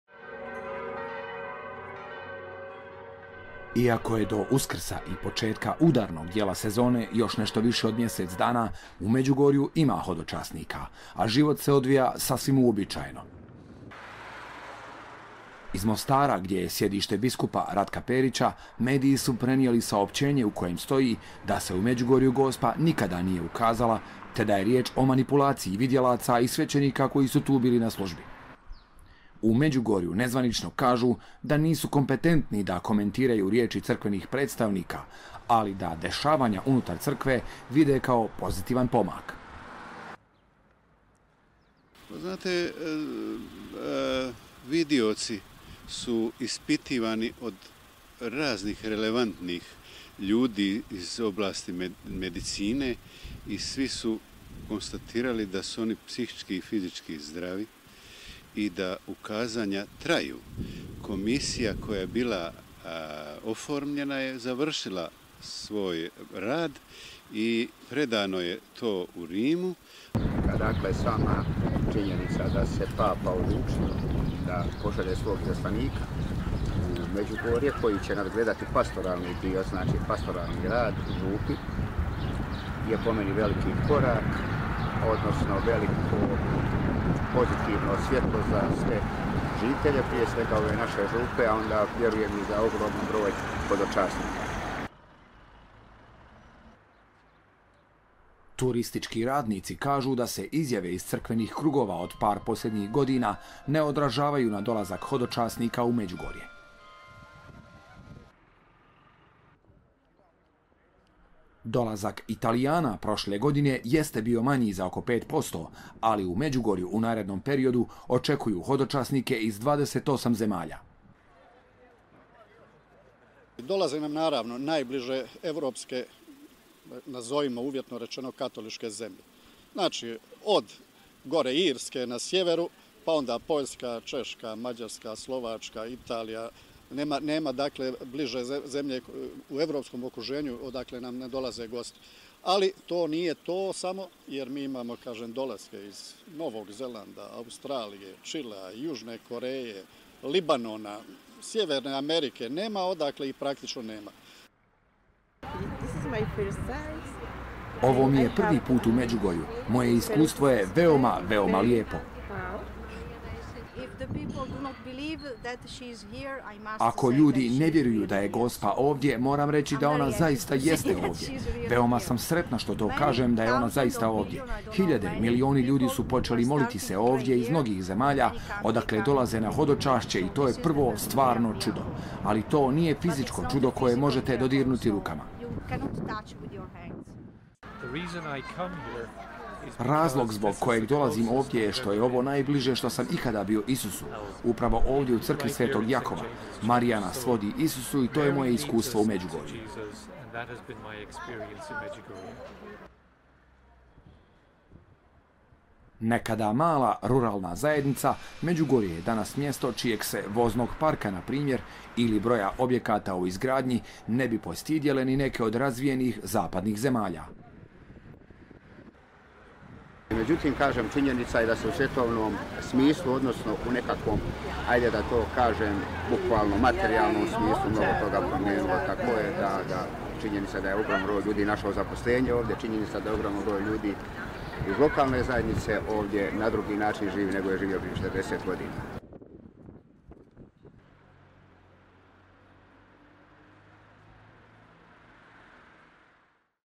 Međugorje: Mit o Gospi i dalje živi
Uprkos tome što je i nadležni mostarski biskup ovih dana ustvrdio kako se Gospa u Međugorju nije ukazala, u tom mjestu i ove godine pripremaju se za početak udarnog dijela sezone, kada će u taj nekada zabačeni kraj masovno stizati rijeke hodočasnika iz cijelog svijeta.